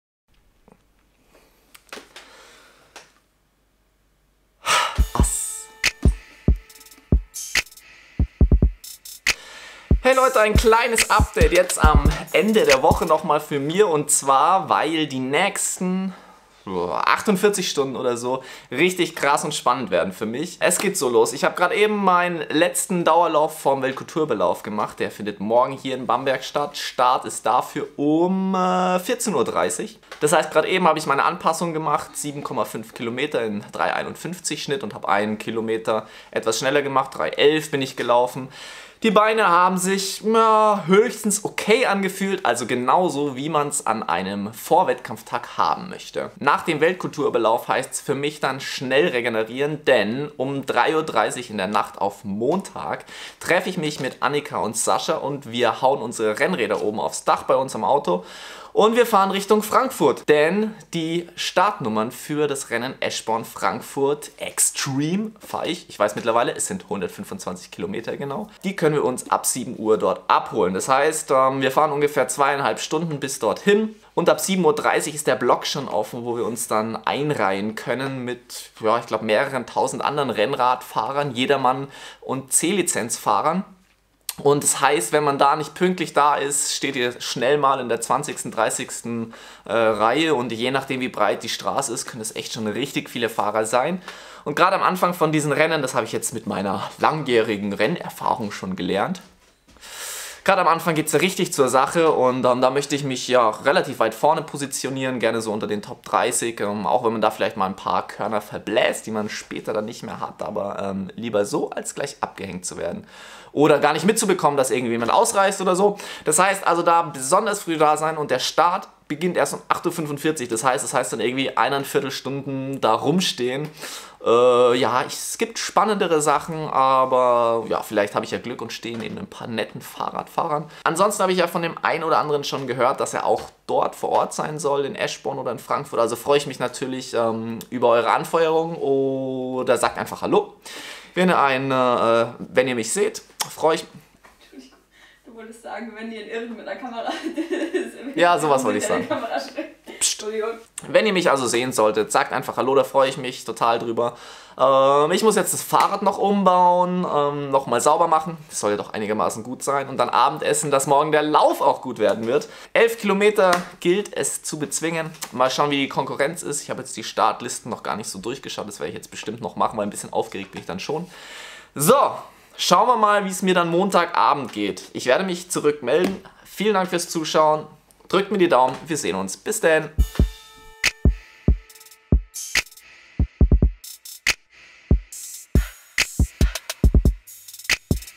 Innerhalb von 24 Stunden: Weltkulturerbelauf und Eschborn-Frankfurt Radrennen
Vlog 97: Heute starte ich beim Welltkulturerbelauf in Bamberg und dann morgen beim Radklassiker Radrennen Eichborn-Frankfurt. Da Alles in weniger als 24 Stunden. Mal sehen, wie das wird.